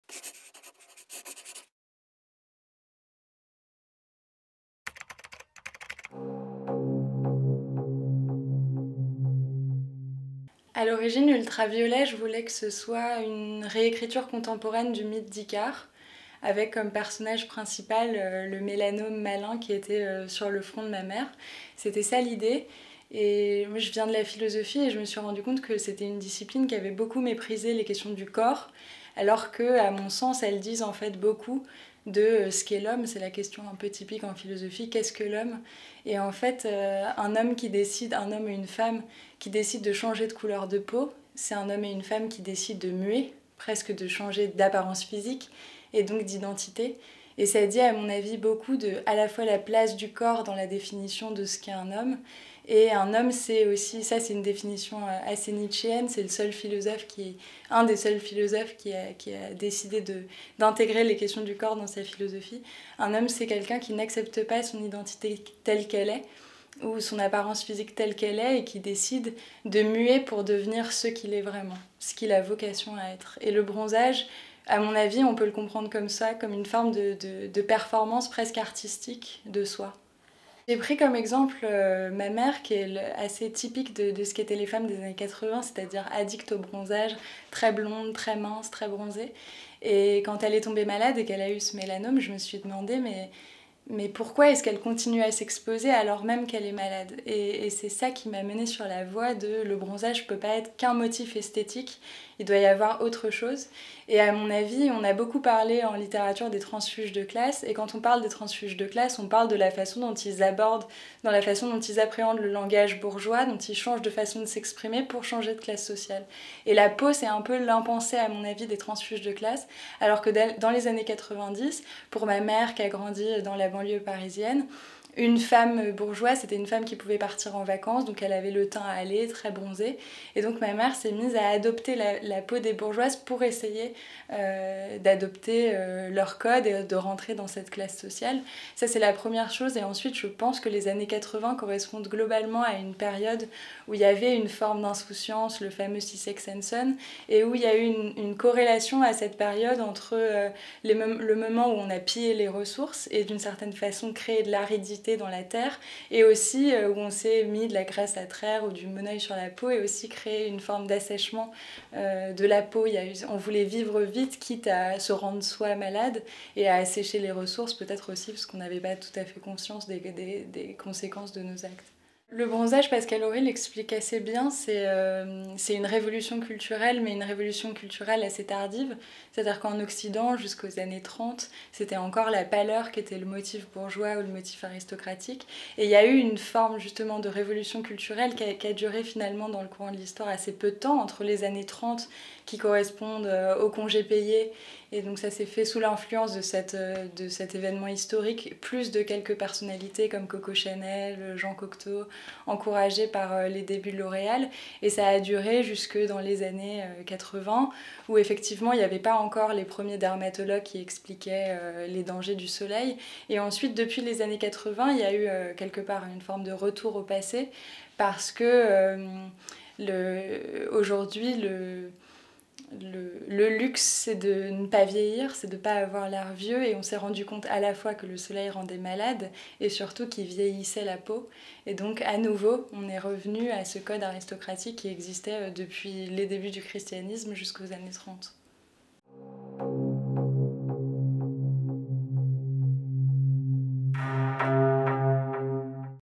Margaux Cassan - Ultra Violet
Margaux Cassan vous présente son ouvrage "Ultra violet" aux éditions Grasset.